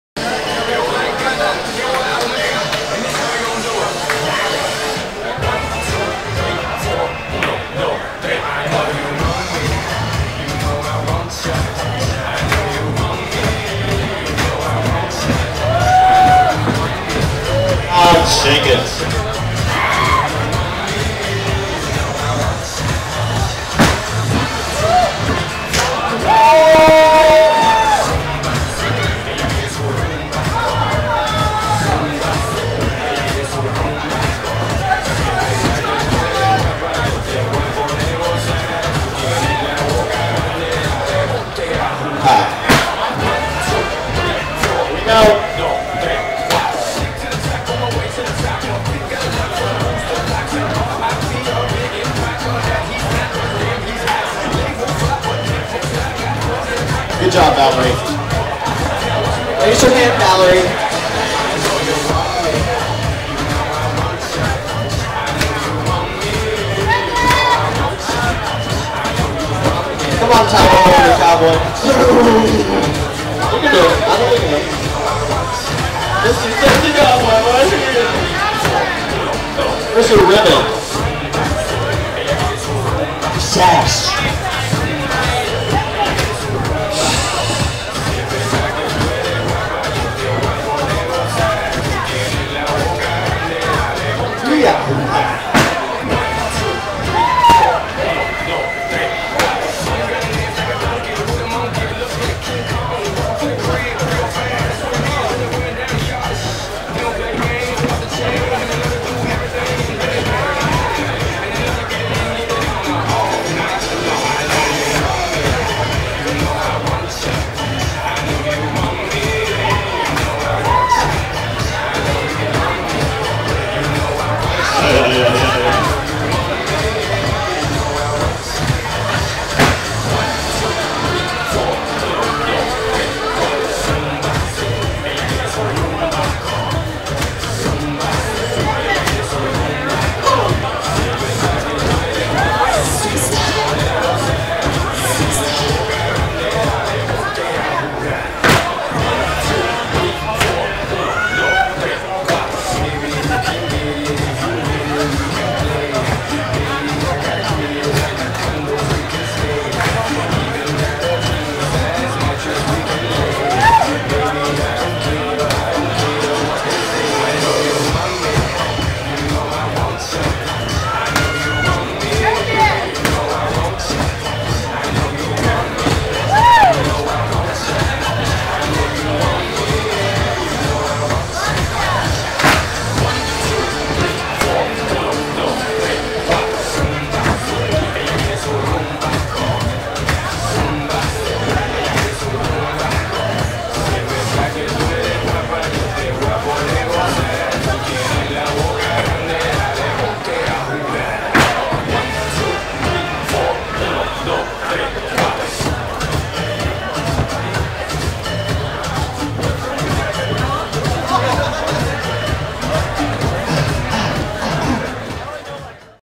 Roomba
Line Dance: Roomba ~ phrased
Choreographer: Guyton Mundy
Thanks "Scandals Saloon Dancers", Wilton Manors, Florida
Steps available on "Kickit"